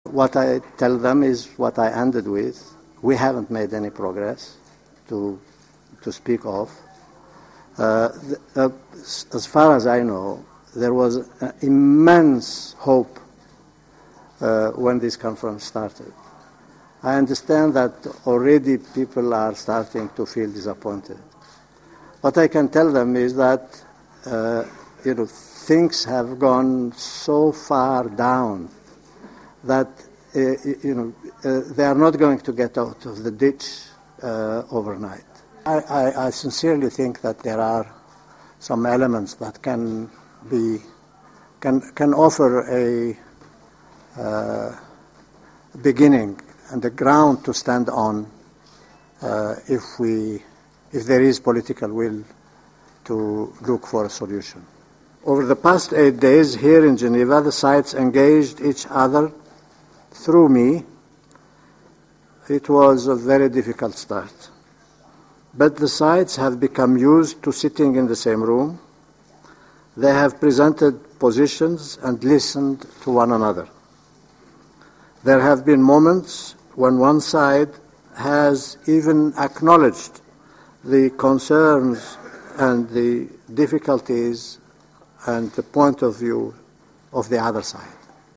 Raw: Brahimi Says No Progress in Syria Talks
After eight days of talks on Syria, UN negotiator Lakhdar Brahimi says there's been no progress, but at least the two sides met at the same time in the same room. (Jan. 31)